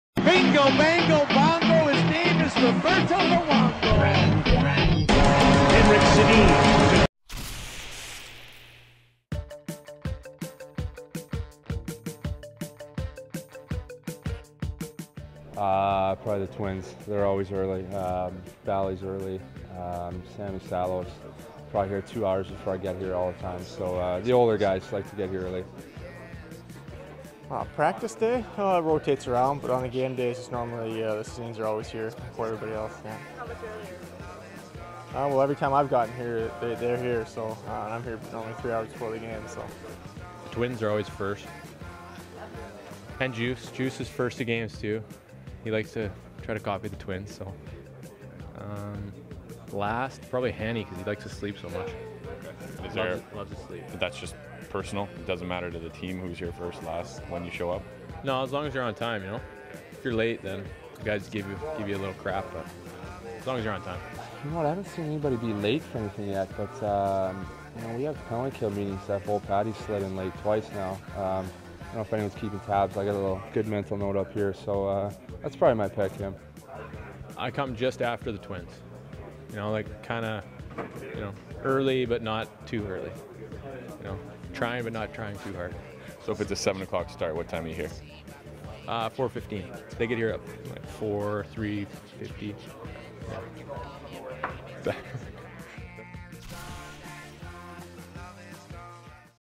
Vancouver Canucks Podcast "On Time"
The canucks tell you who's on time and who's late for the bus